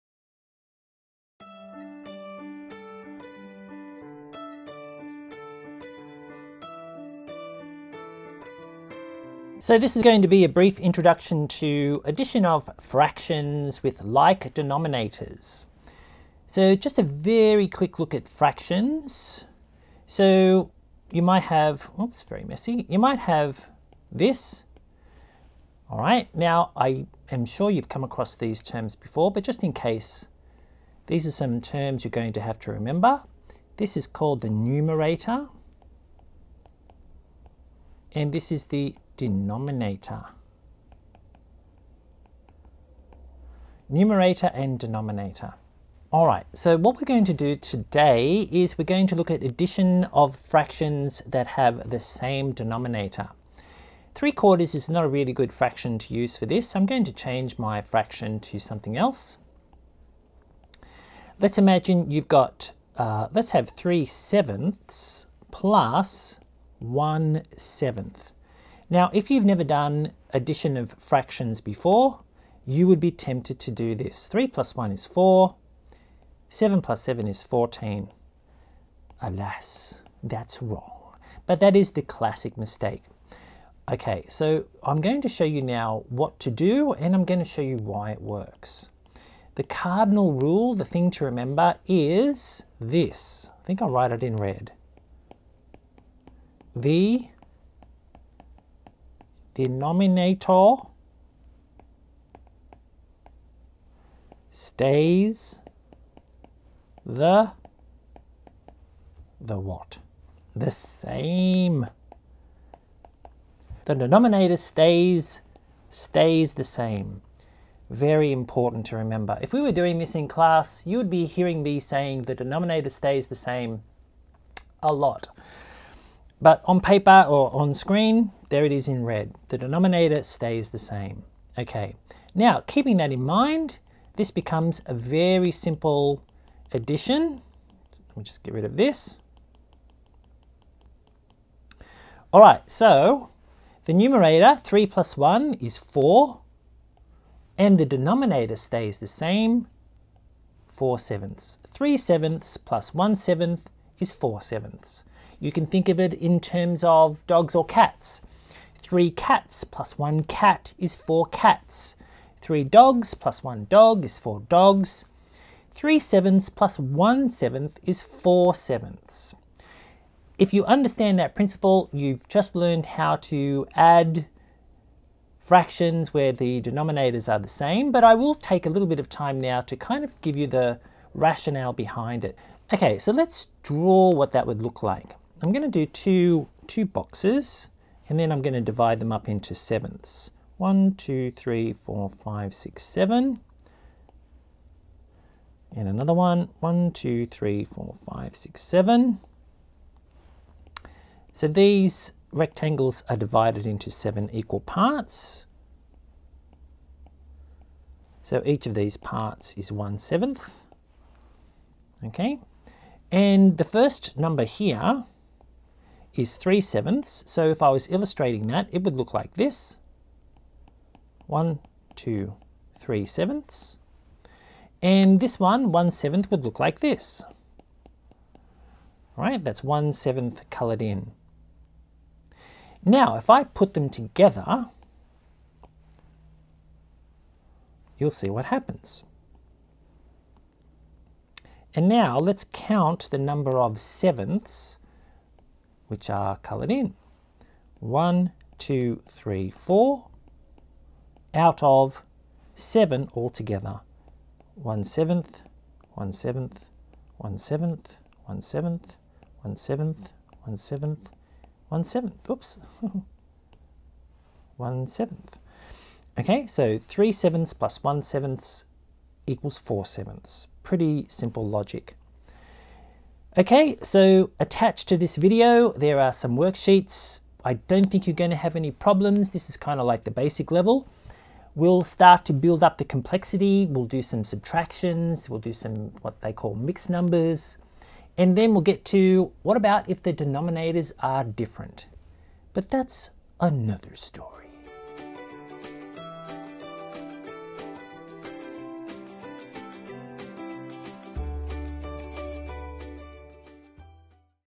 Fractions: Adding Like Denominators
The favorite kind of fractions question! Super-easy but with a common error that is easy to avoid. This video shows how to do it and briefly explains the rationale.

Made for Glenwood’s Google Classroom remote learning, the video may refer to specific content such as activities or content but works just as well by itself as a stand-alone tutorial for revision, extra explanations or independent learning.